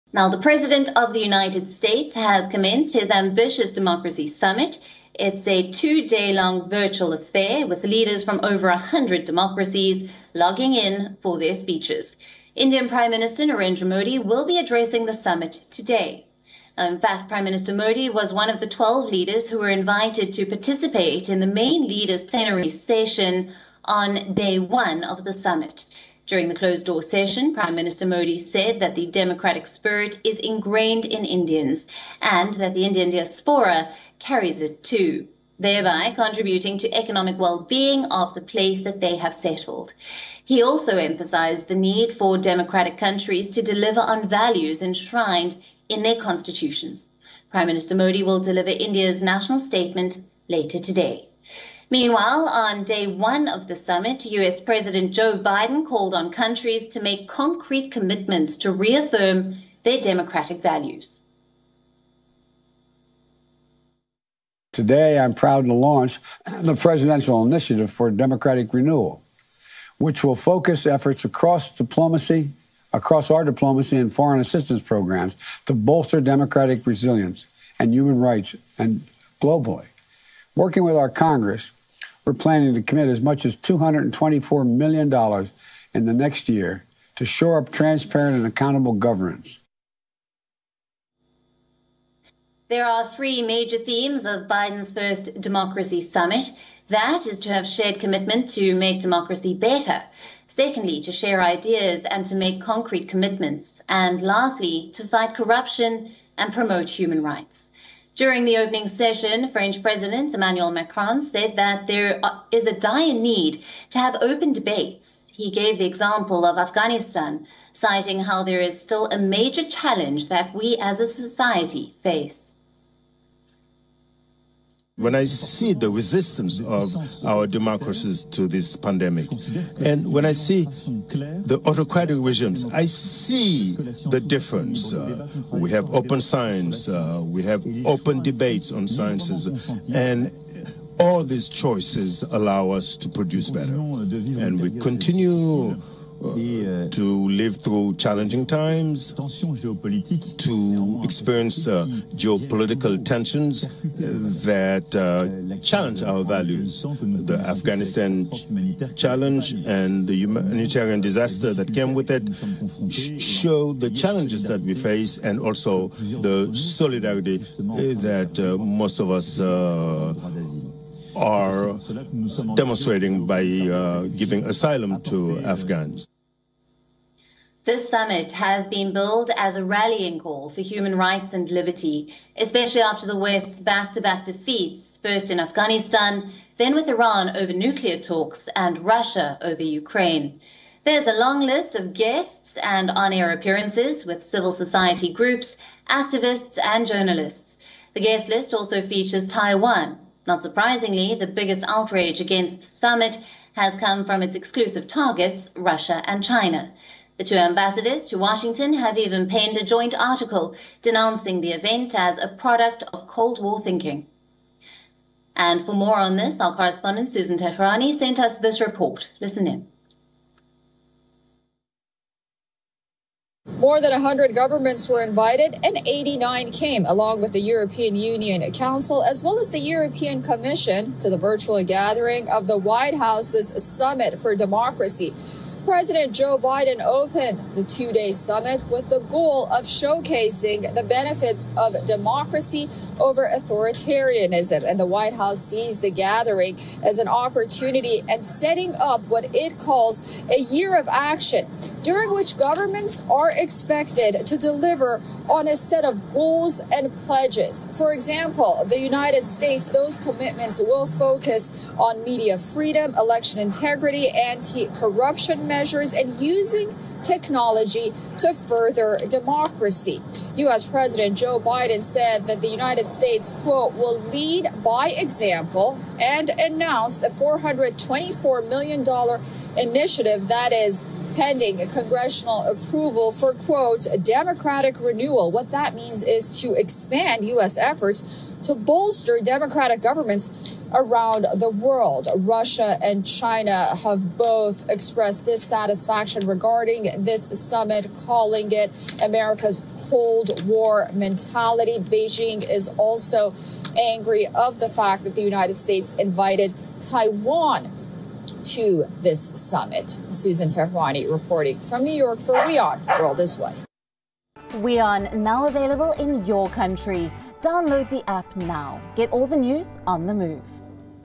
India's PM Narendra Modi to address US President Joe Biden's virtual Democracy Summit | English News
Indian Prime Minister Narendra Modi will address US President Joe Biden's virtual Democracy Summit. Over 100 nations are in attendance.